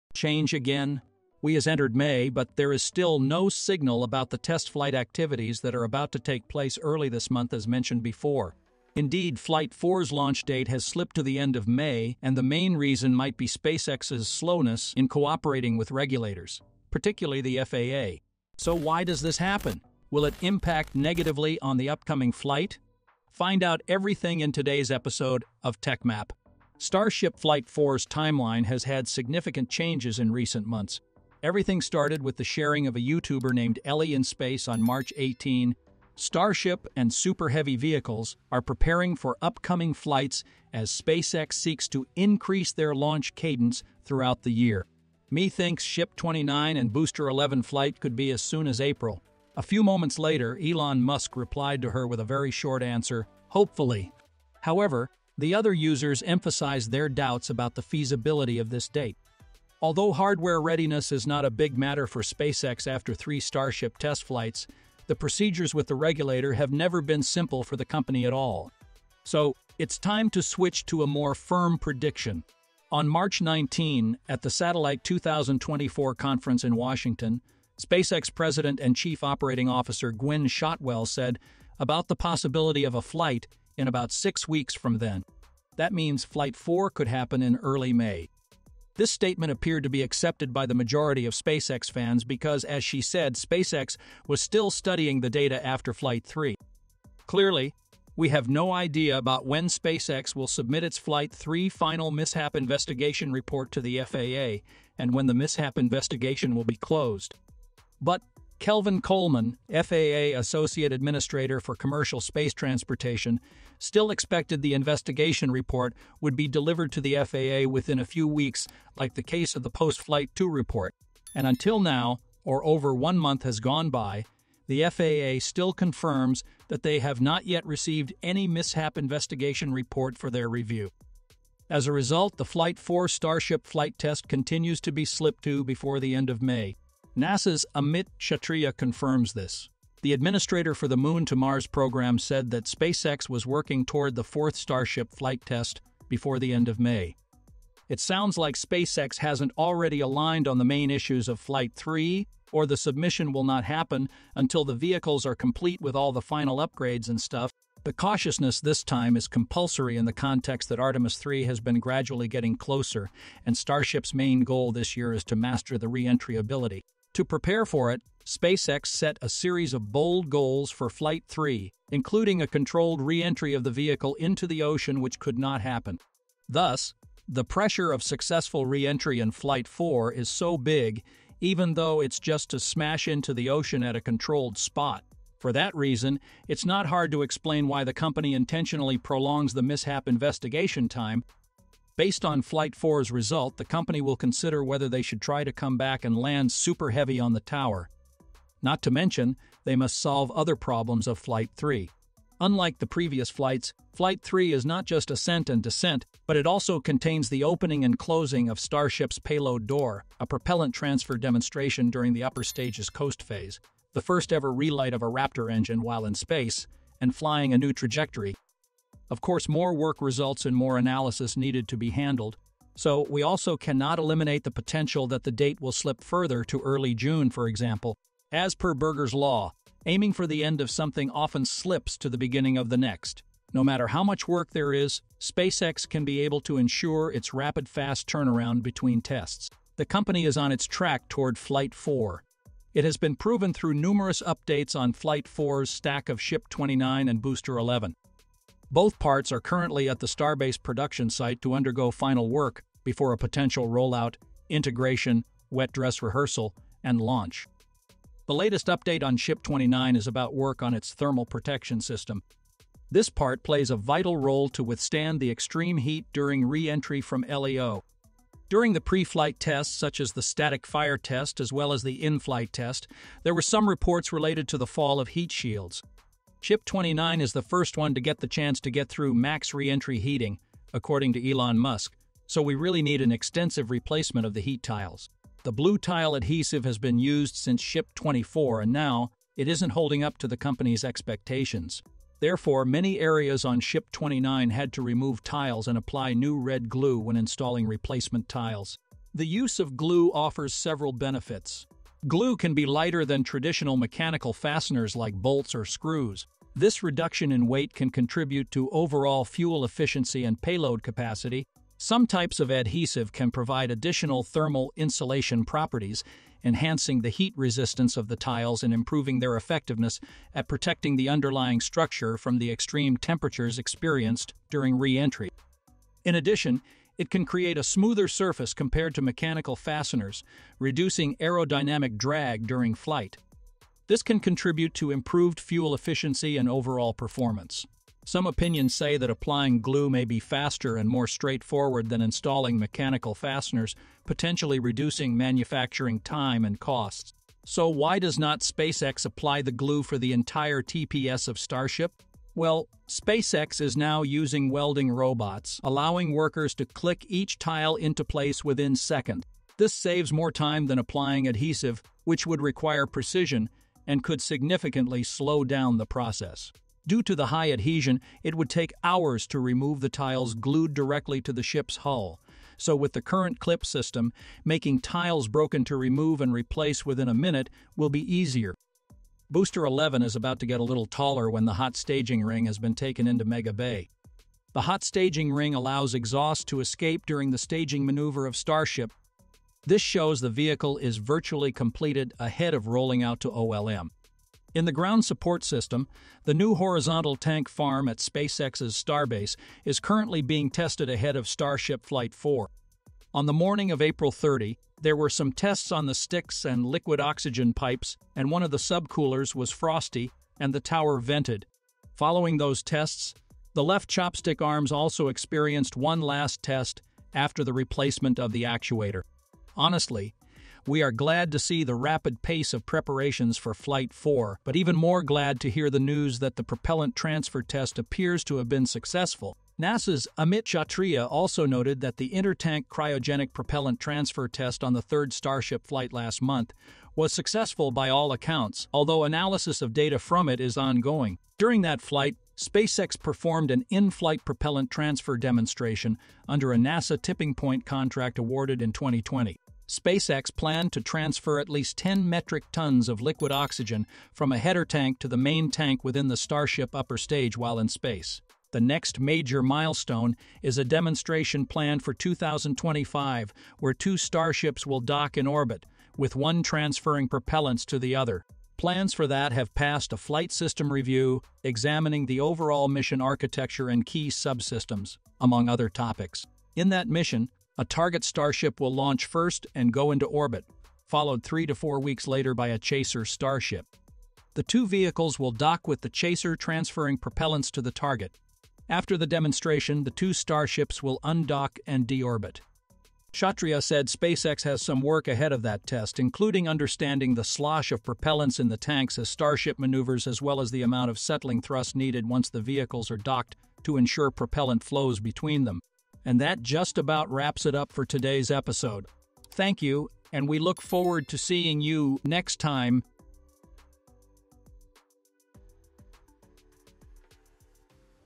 Nasa just announced new launch date on SpaceX Starship Flight 4!
(Intro) 0:00
(The previous announcements about Flight 4’s launch date) 0:26
(New launch date on Starship Flight 4) 2:11
(The propellant transfer test on Starship) 6:59
(Outro) 8:25
1) SOURCES OF THUMBNAIL
Change again!
We has entered May, but there is still no signal about the test flight activities that are about to take place early this month as mentioned before.
Indeed, Flight 4's launch date has slipped to the end of May and the main reason might be SpaceX's slowness in cooperating with regulators, particularly the FAA.
So, why does this happen?
Will it impact negatively on the upcoming flight?
Find out everything in today's episode of Techmap.
(The previous announcements about Flight 4’s launch date)
Starship Flight 4's timeline has had significant changes in recent months.
Everything started with the sharing of a YouTuber named Ellie in Space on March 18:
"Starship and Super Heavy vehicles are preparing for upcoming flights as SpaceX seeks to increase their launch cadence throughout the year.
Me thinks Ship 29 and Booster 11 flight could be as soon as April."
A few moments later, Elon Musk replied to her with a very short answer:
"Hopefully".